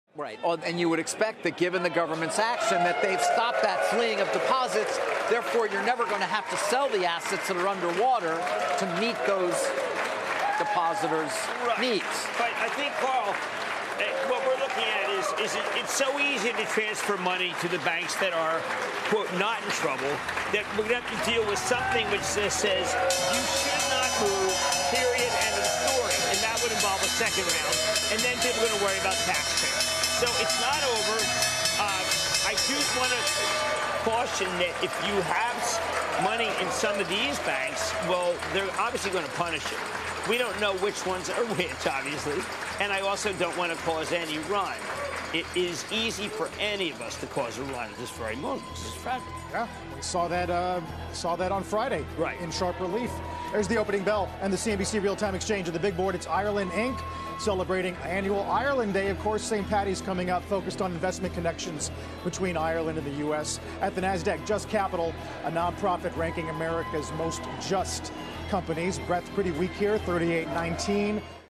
Opening Bell: March 13, 2023
Watch the opening bell for the Nasdaq and the New York Stock Exchange from March 13, 2023.